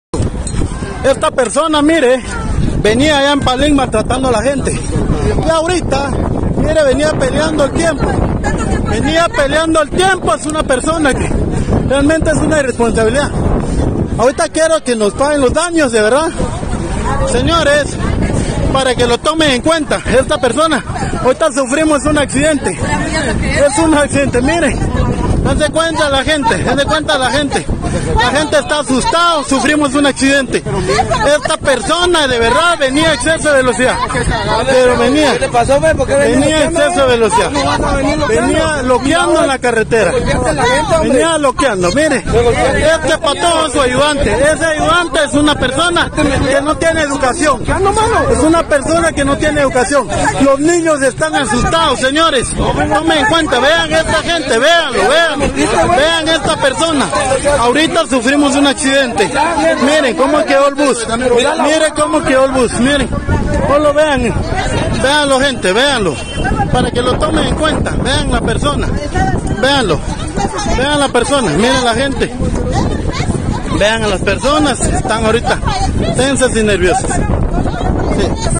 Autobús se accidenta en la ruta al Pacífico, pasajeros reclaman al conductor
Un accidente de tránsito ocurrió este jueves 8 de noviembre en el kilómetro 39 de la ruta al Pacífico. El siniestro vial fue protagonizado por un autobús de la empresa Esmeralda, el cual colisionó en contra de la barrera metálica protectora de la referida vía.

Por medio de un video que fue difundido a través de las redes sociales se observa el estado del vehículo de transporte de pasajeros, además de cómo los usuarios descienden de la unidad y le reclaman al conductor por manejar de manera imprudente.

Video: Vía Víctor Bolaños